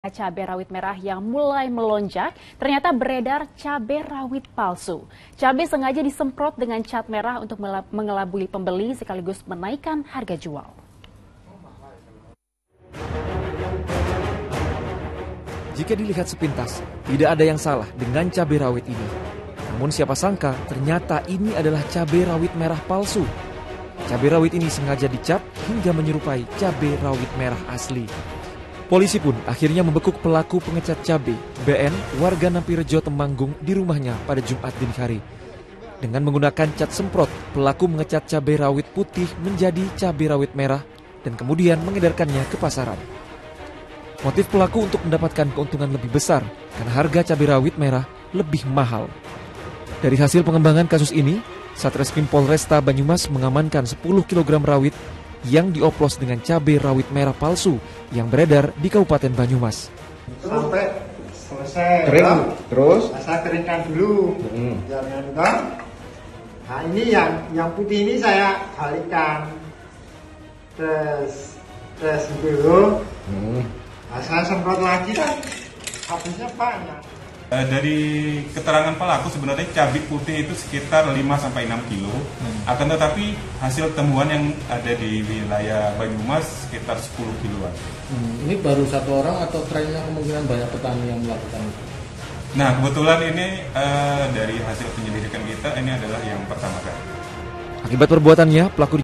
Polisi Bekuk Pelaku Pengecat Cabai Rawit Hijau Jadi Warna Merah | Liputan 6
Jajaran Reskrim Polresta Banyumas bekerjasama dengan Polres Temanggung akhirnya membekuk pelaku pengecat cabai rawit merah yang sempat menghebohkan warga Banyumas. pelaku ternyata seorang petani cabai yang mengecat cabai itu dengan cat semprot.